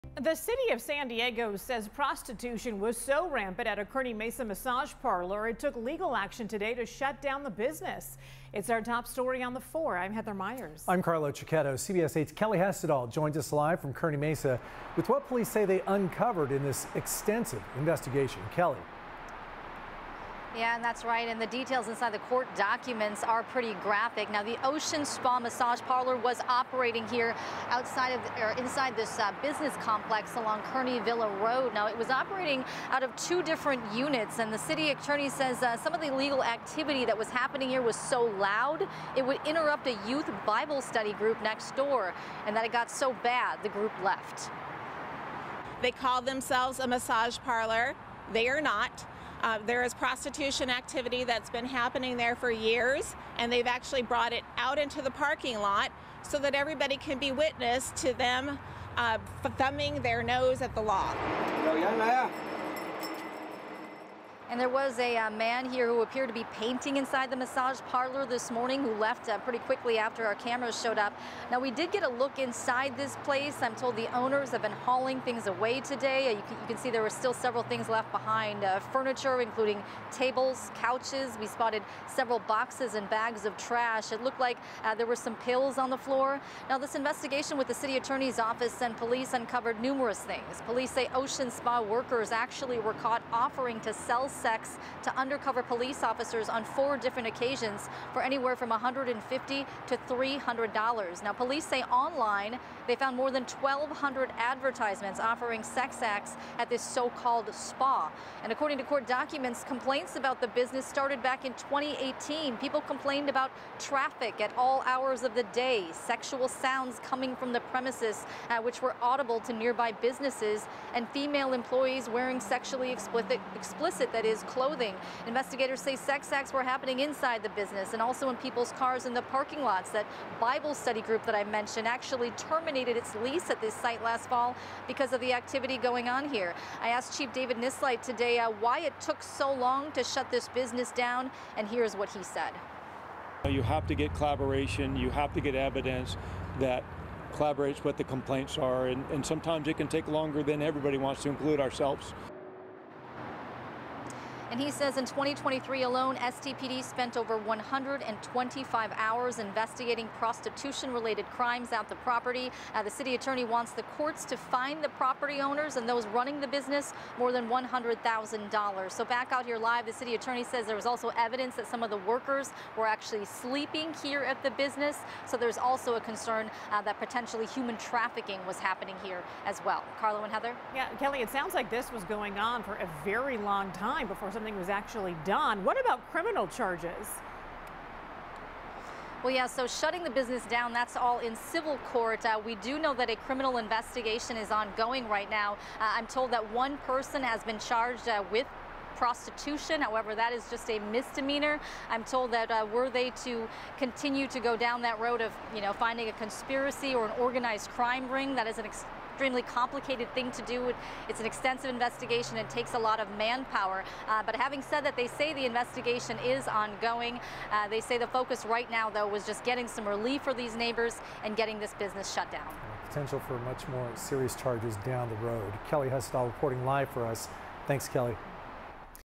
City of San Diego takes legal action to shut down Ocean Spa Massage Parlor in Kearny Mesa
Police said some of the illegal acts that were going on there were so loud, they actually disrupted a Bible youth study group.